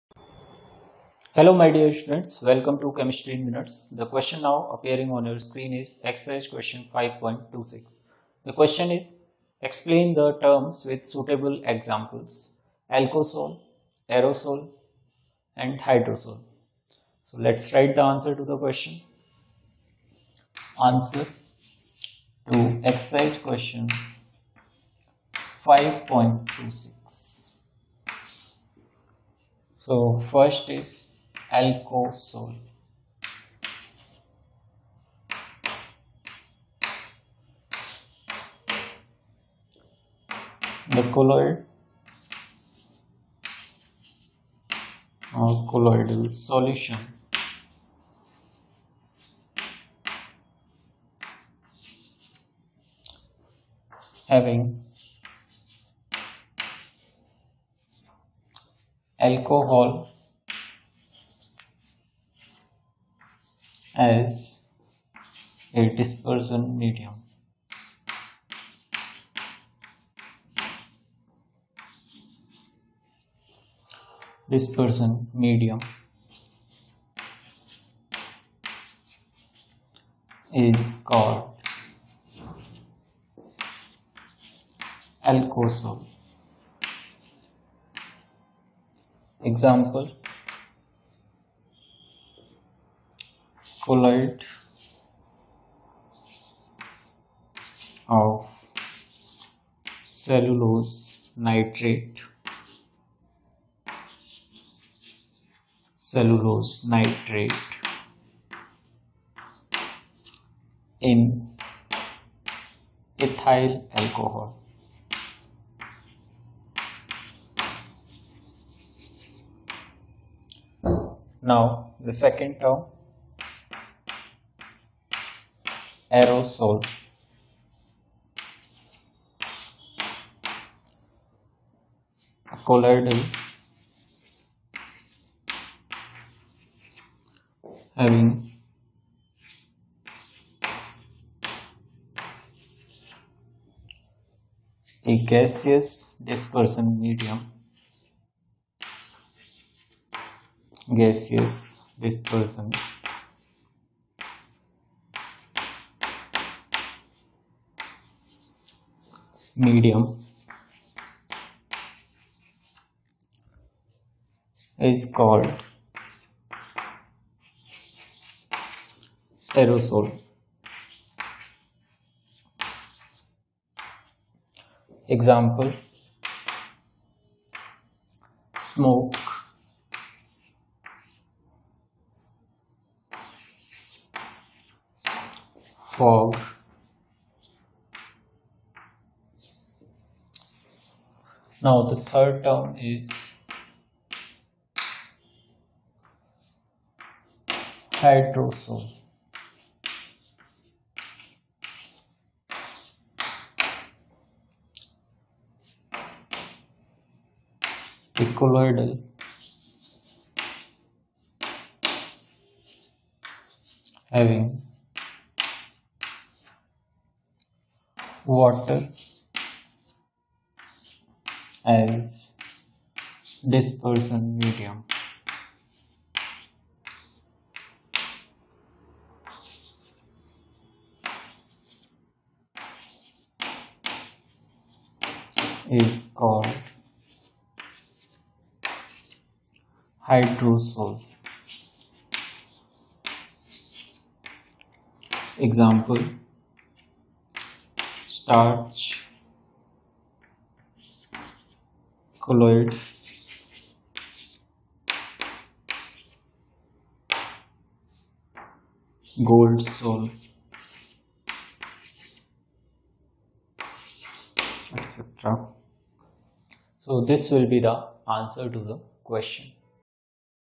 SURFACE CHEMISTRY E.Q.5.26 CLASS 12 CHEMISTRY NCERT CHAPTER 5
Explain the terms with suitable examples:
(i) Alcosol                       (ii) Aerosol         (iii) Hydrosol.
Solution to NCERT Exercise Question chemistry class 12 CHAPTER 5 SURFACE CHEMISTRY Exercise Question (E.Q.) 5.26